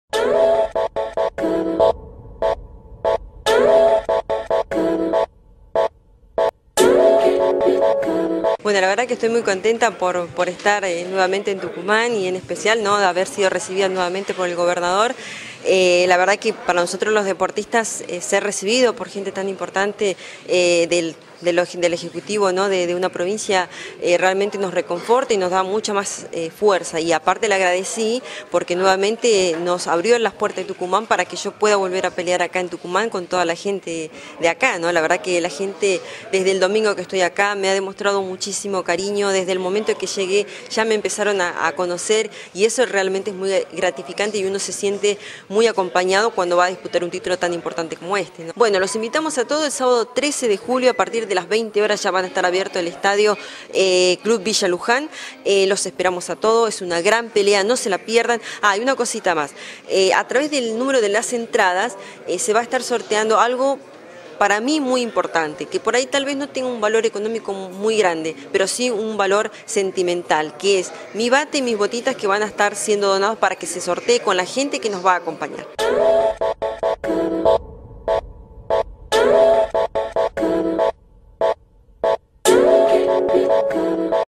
Boxeo Internacional en Tucumán
El Gobernador de la provincia, José Alperovich, recibió en su despacho a la boxeadora y campeona mundial Marcela "La Tigresa" Acuña.
La deportista formoseña engalanara la provincia en una muestra del mejor boxeo de latinoamerica y el mundo, enfrentando a la puertorriqueña Melissa Hernández, el próximo sábado en el Club Villa Lujan de esta Capital
Estoy agradecida de poder pelear nuevamente en Tucuman, una provincia donde se trabaja en lo cotidiano para la inclusión social y el desarrollo de las actividades deportiva que permiten alejar a los mas jóvenes de la calle y las drogas, manifestó Acuña